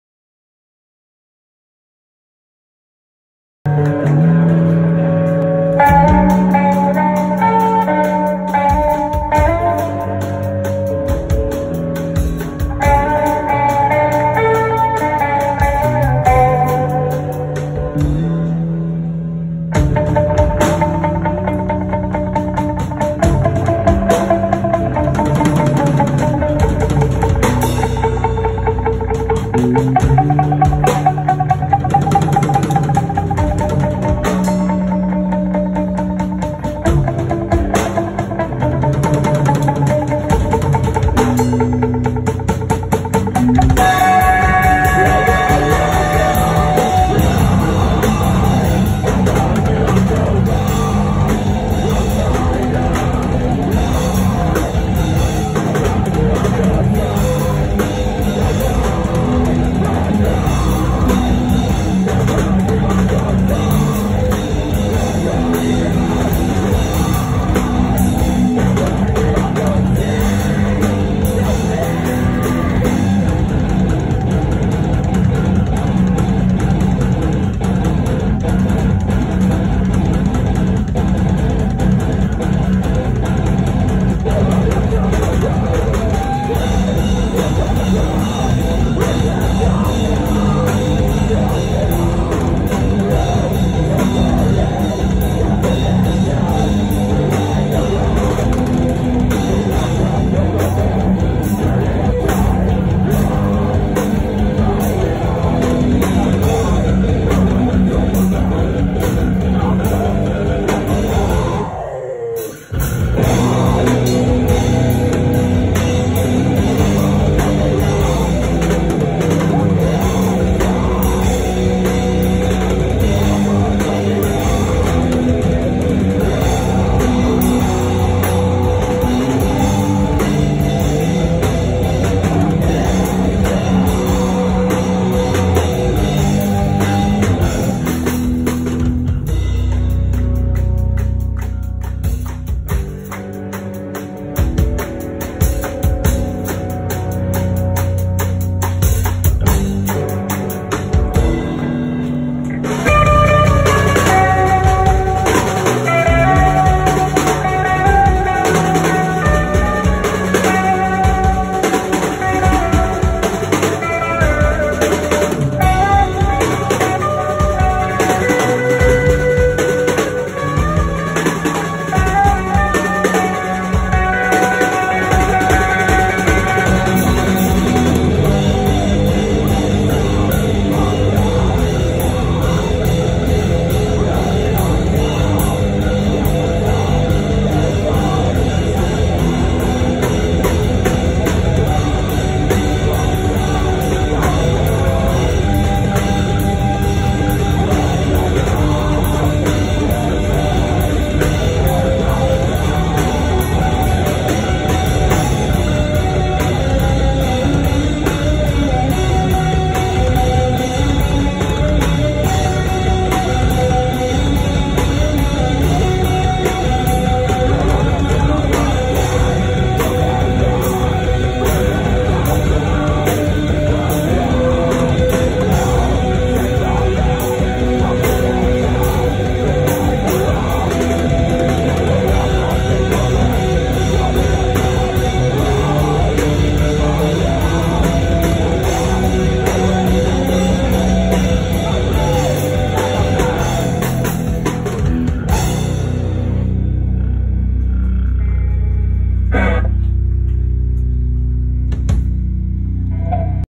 13Years - Drum Camp Live Innocent Fest Kusus
Video dokumentasi dari akun YouTube @berbagiceritanyata
saat live di kudus Jawa Tengah